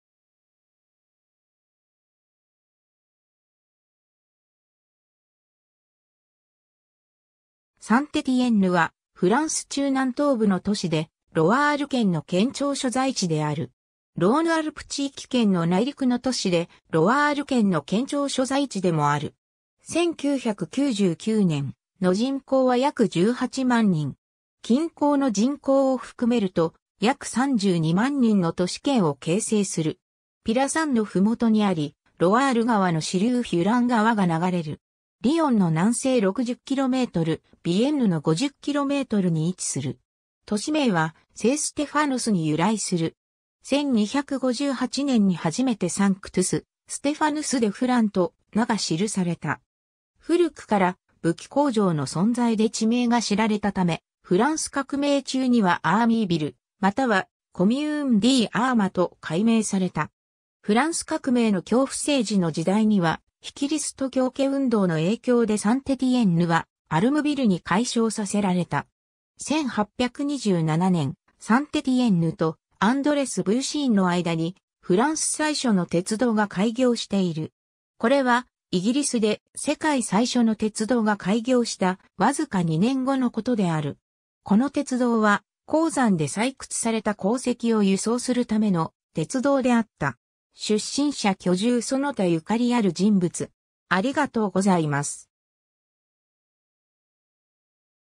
サン＝テティエンヌ
サン＝テティエンヌ（Saint-Étienne）は、フランス中南東部の都市で、ロワール県の県庁所在地である。
 ローヌ＝アルプ地域圏の内陸の都市で、ロワール県の県庁所在地でもある。
1999年の人口は約18万人。
近郊の人口を含めると約32万人の都市圏を形成する。
 ピラ山の麓にあり、ロワール川の支流フュラン川が流れる。
リヨンの南西60km、ヴィエンヌの50kmに位置する。
 都市名は聖ステファヌスに由来する。
1258年に初めてSanctus Stephanus de Furan（フュラン川の聖ステファヌス）と名が記された。
古くから武器工場の存在で地名が知られたため、フランス革命中にはArmeville（武器の街）またはCommune d'Armes（武器のコミューン）と改名された。
 フランス革命の恐怖政治の時代には、非キリスト教化運動の影響でサン＝テティエンヌはアルムヴィル（武装せる都市）に改称させられた。
 1827年、サン＝テティエンヌとAndrézieux-Bouthéon（サン＝テティエンヌの北西約15kmの町で、サン＝テティエンヌ空港の近くである）の間に、フランス最初の鉄道が開業している。
これは、イギリスで世界最初の（営業用）鉄道が開業した、わずか2年後のことである。
この鉄道は、鉱山で採掘された鉱石を輸送するための鉄道であった。
 出身者 居住その他ゆかりある人物